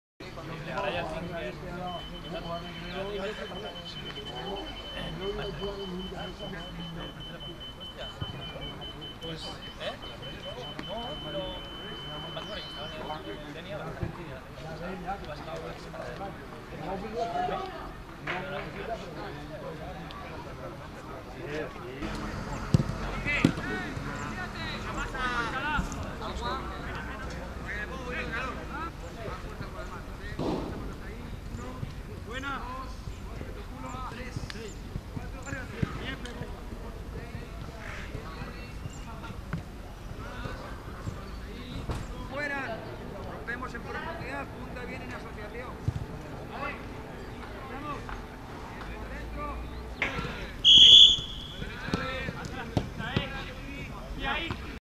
Así se prepara Alphonse Aréola, el portero titular del Villarreal CF
Entrenamiento del Villarreal del 4/9/2015